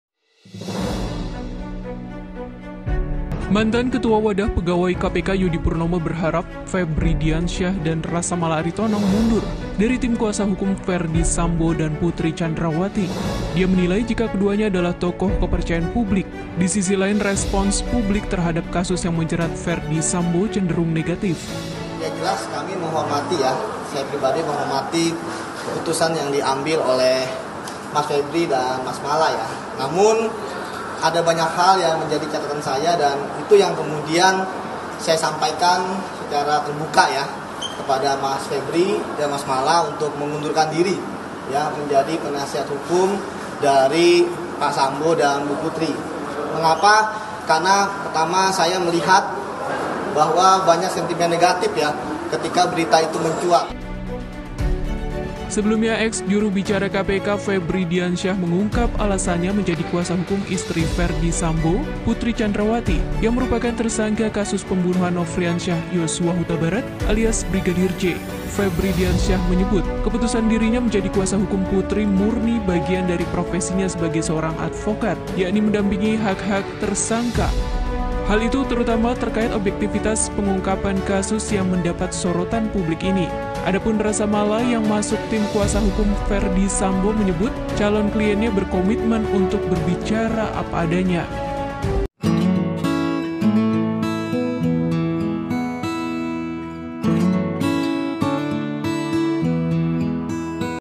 Sisi Lain Febri Dan Rasamala Adalah Tokoh Kepercayaan Publik
Bekas Ketua Wadah Pegawai KPK Yudi Purnomo Mengharapkan Agar Febri Diansyah Dan Rasamala Aritonang Mundur Sebagai Pengacara Istri Ferdy Sambo Putri Candrawathi, Karena Respons Publik Terhadap Kasus Tersebut Cenderung Negatif.